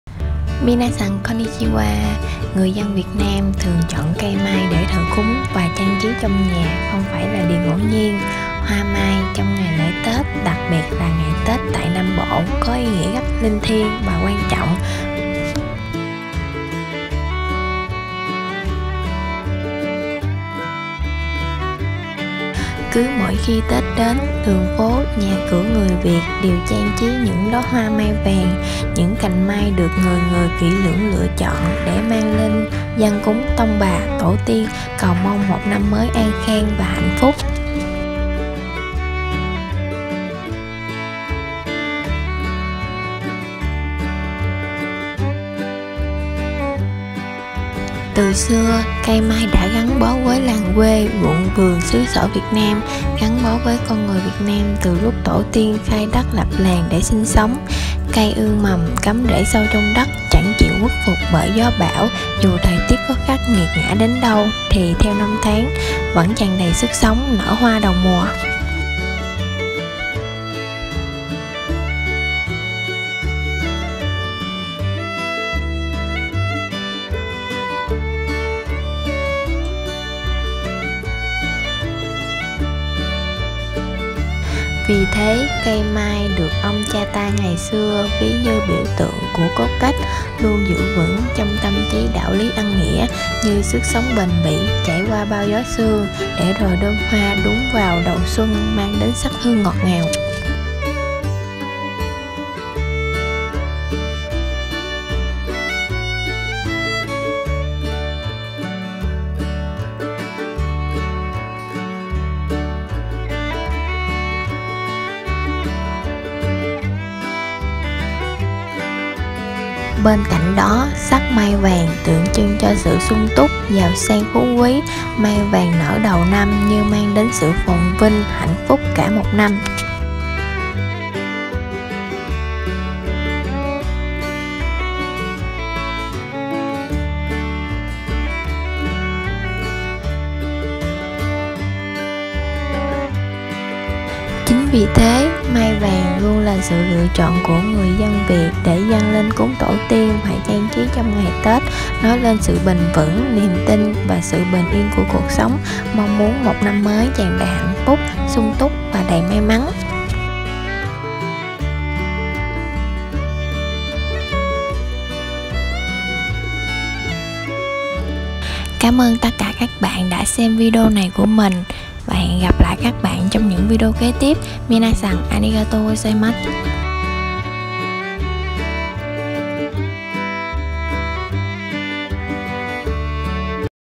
Hoa mai ngày tết - Apricot flowers on Tet Holiday
Mỗi dịp tết đến xuân về, nhà nhà đều trưng bày những chậu mai rực rỡ để cầu mong một năm mới cho gia đình may mắn, bình an và thịnh vượng.